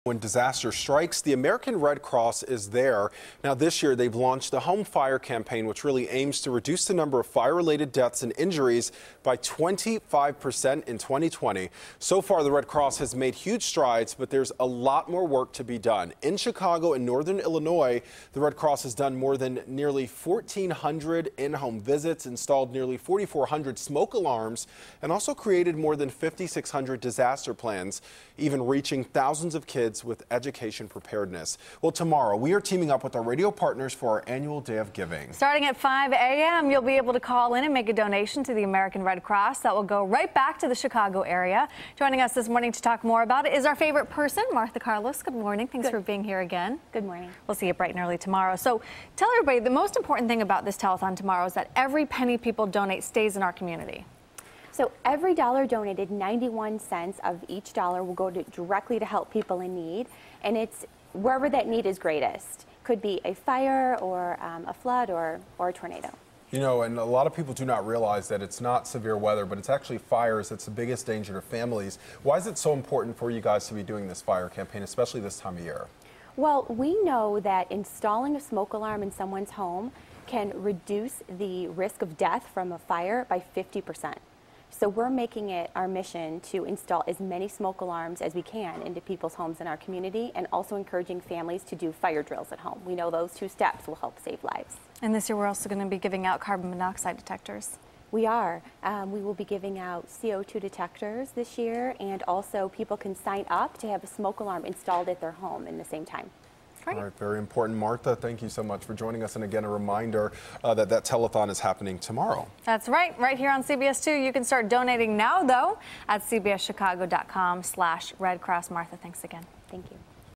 Red Cross Launching Home Fire Campaign
When disaster strikes, the American Red Cross is there. This year, they've launched the "Home Fire" campaign, which aims to reduce the number of fire-related deaths and injuries by 25 percent by 2020.